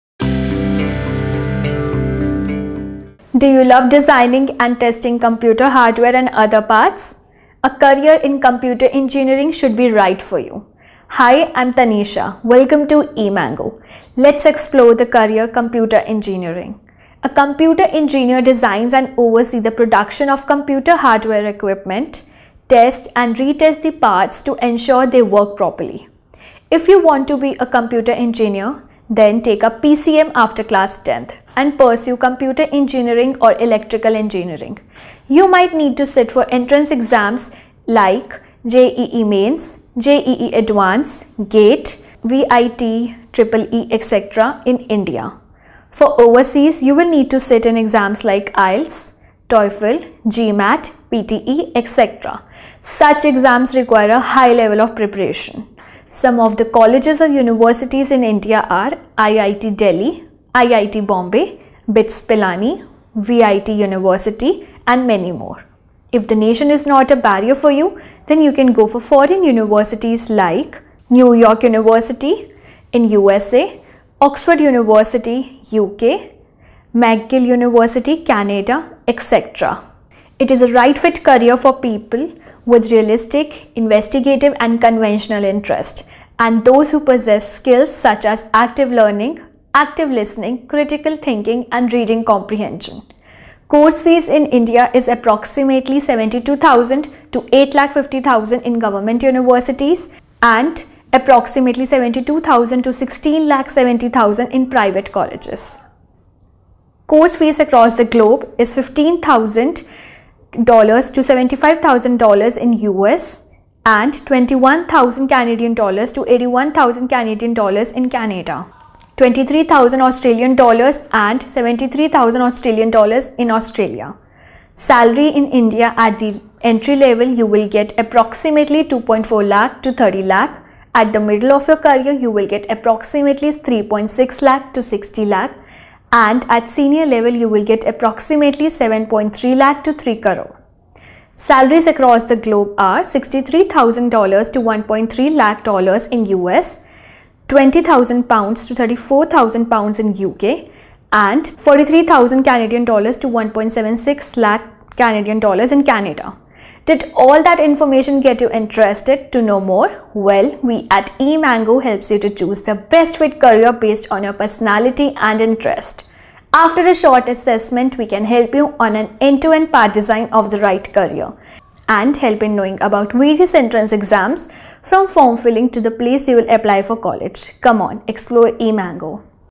Computer Engineer
Computer Engineers, also called computer hardware engineers, are the people accountable for designing and testing your laptop and desktop computers.